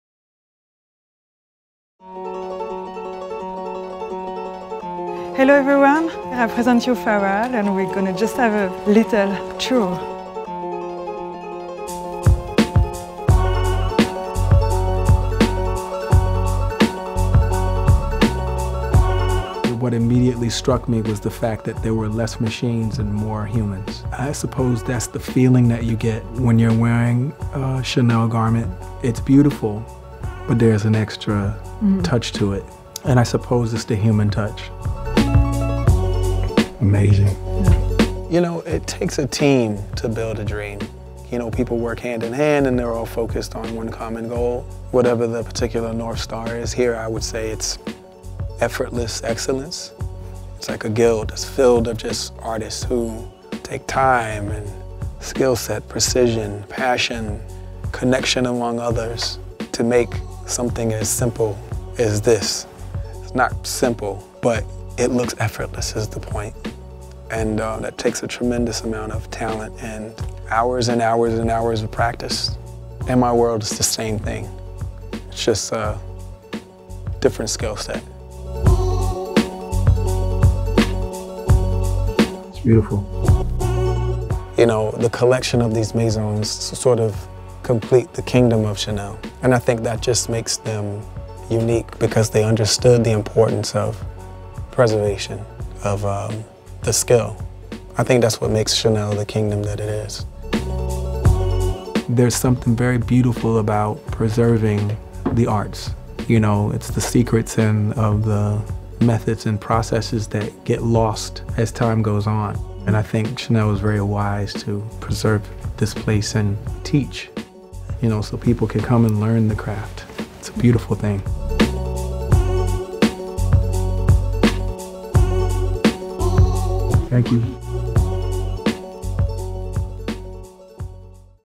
The Paris — 31 rue Cambon 2019/20 Métiers d’art collection - CHANEL
Follow Pharrell Williams in the ateliers of the embroiderer and tweed maker House of Lesage, hatmaker House of Michel and feather and flower maker House of Lemarié for System Magazine around the moment of the Paris — 31 rue Cambon 2019/20 Métiers d’art collection creation.